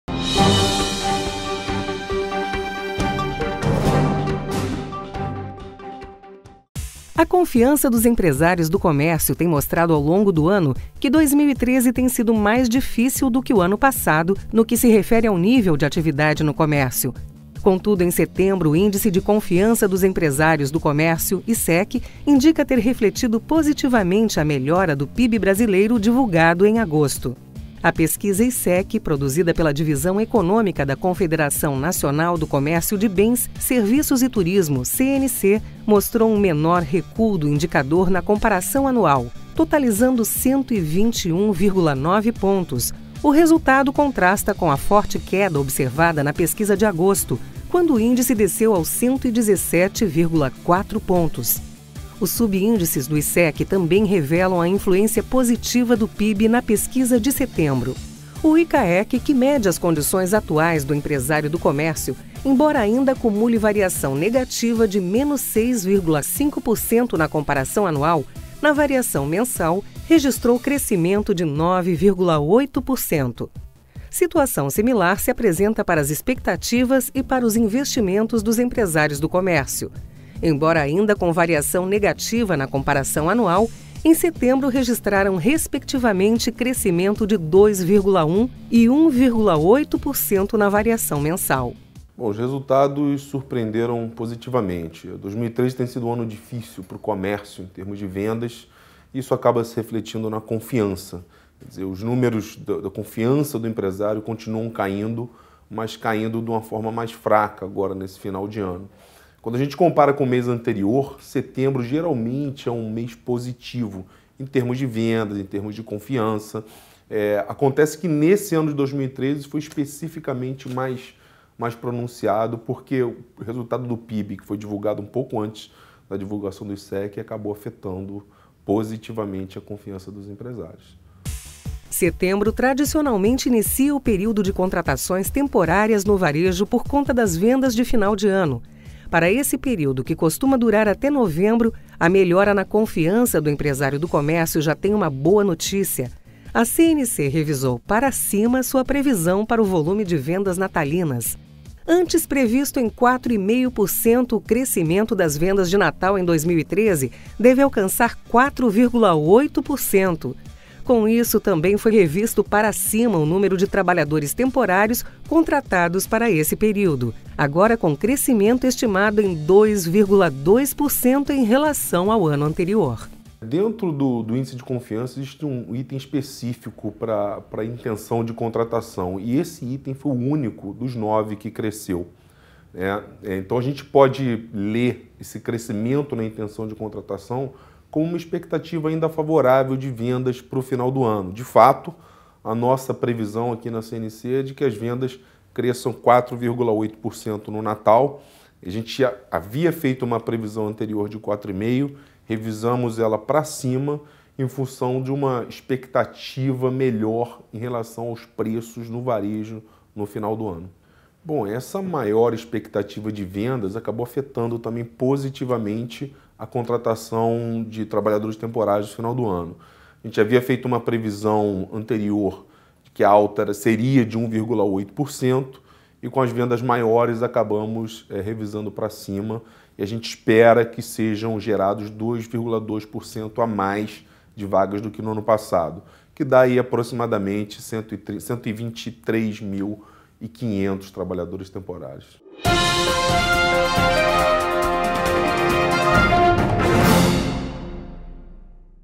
PIB mais favorável eleva confiança do comércio
Índice de Confiança do Empresário do Comércio (ICEC) apresenta alta em setembro, um reflexo direto da melhora do PIB no segundo trimestre. Isso influencia a expectativa das vendas de fim de ano, que devem gerar cerca de 123 mil vagas de trabalho temporário.